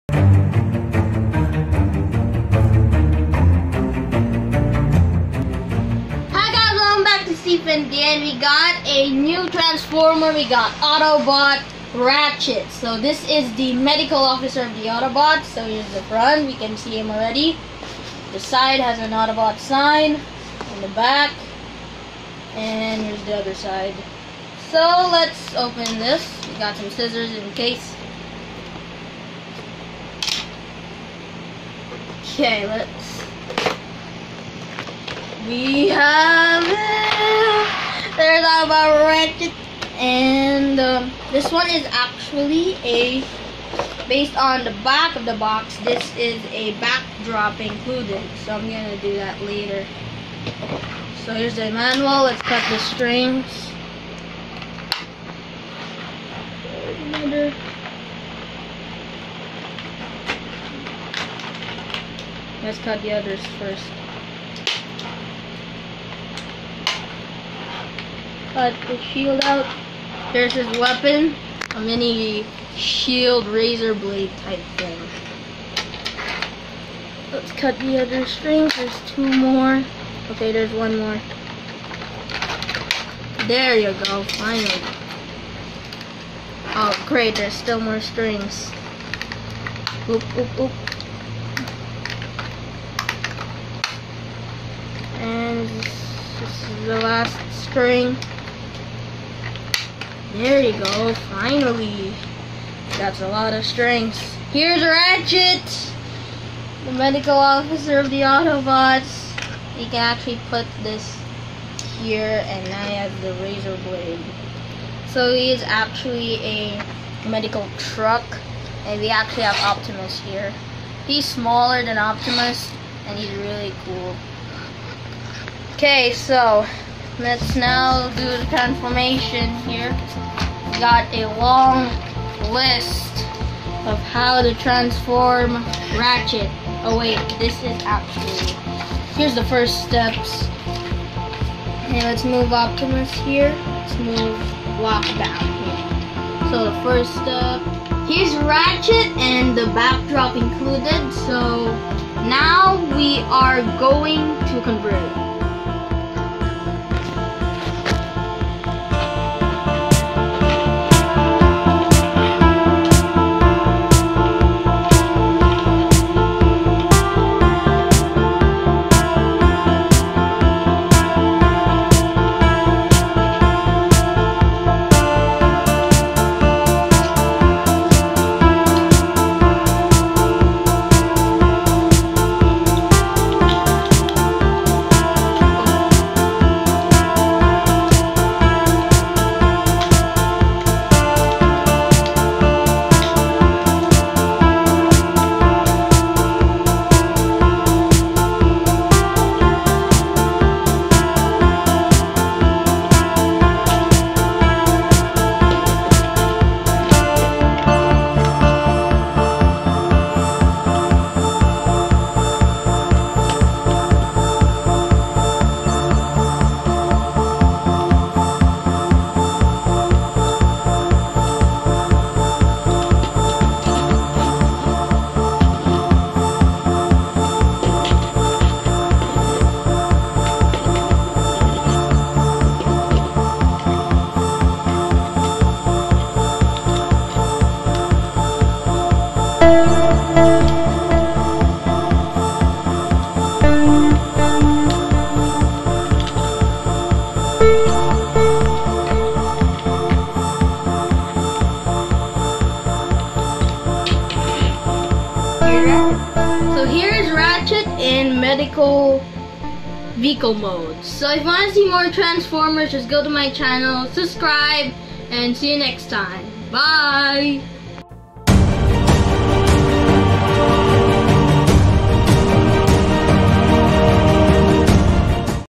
STEPHEN Transforms Ratchet
A reward from dad which I enjoyed transforming. On my way to 200!